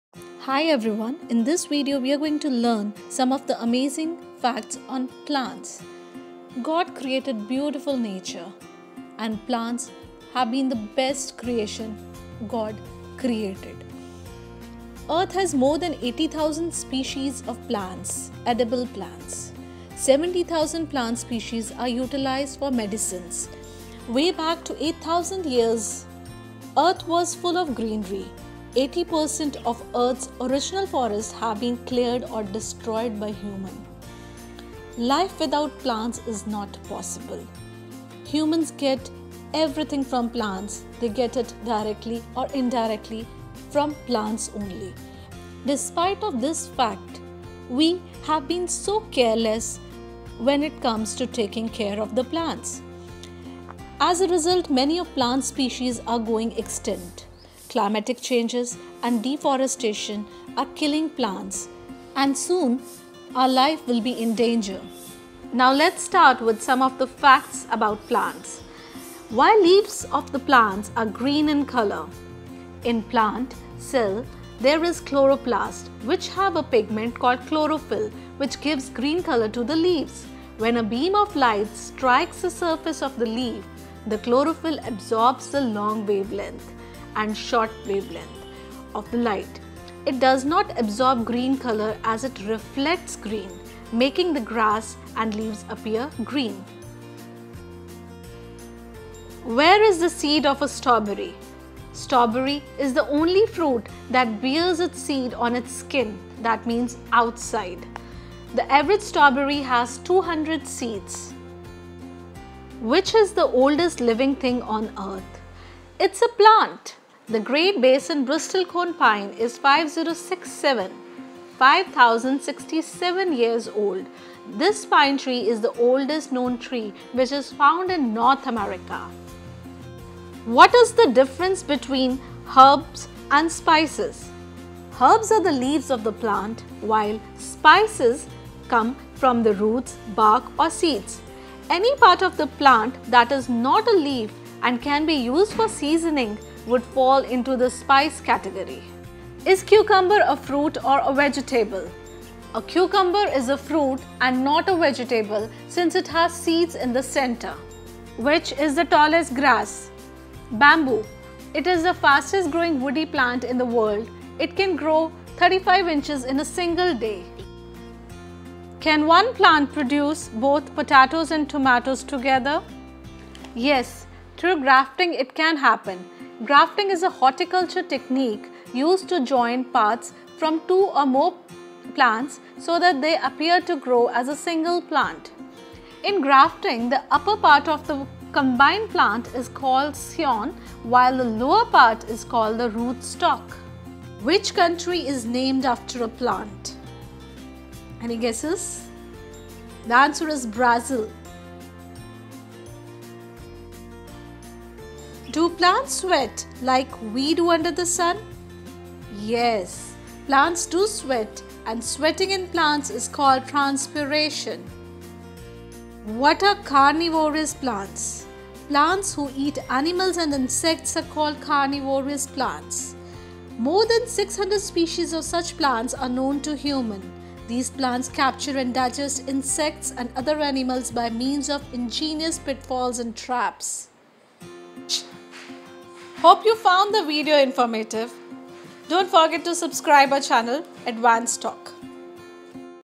Plant quiz- Amazing plant facts
Facts about plants, rare facts about plants, plants quiz.
Hey Guys lets check how much you know about plants( carnivores plants , fruits, vegetables, planting techniques).